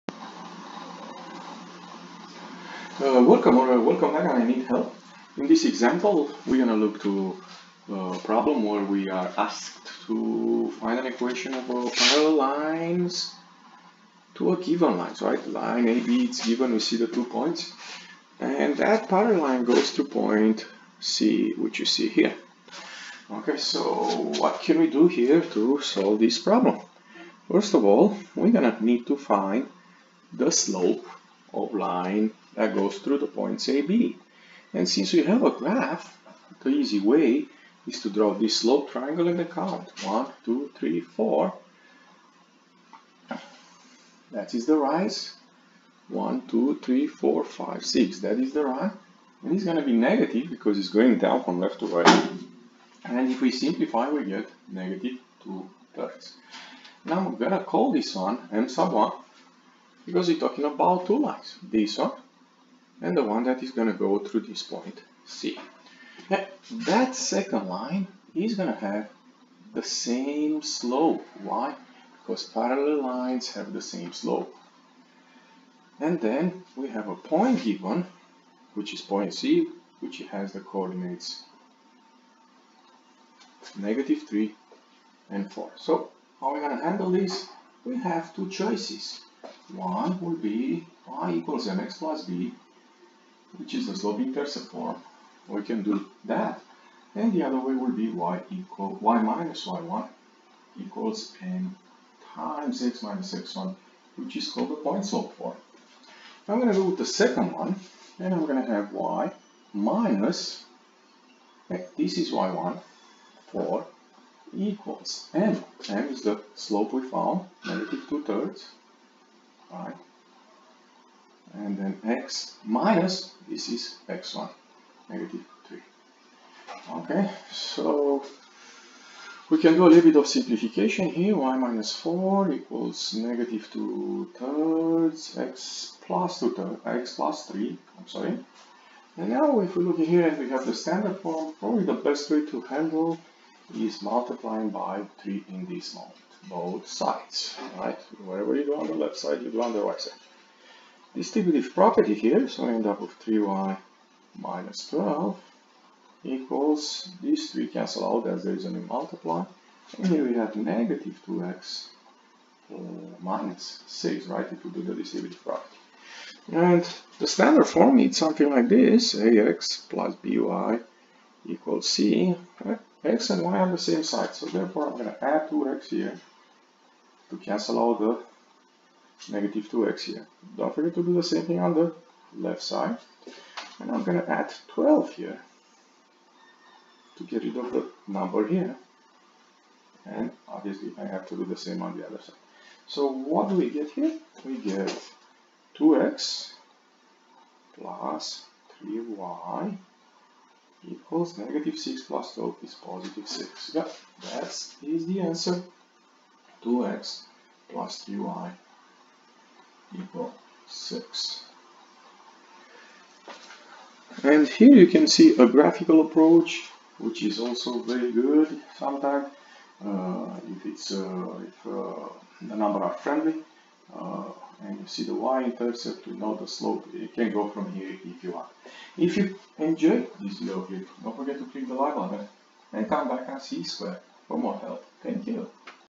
INH: Writing Equations of Lines: Parallel Lines #1
This is an example of writing equations of a parallel line on a given line through a given point.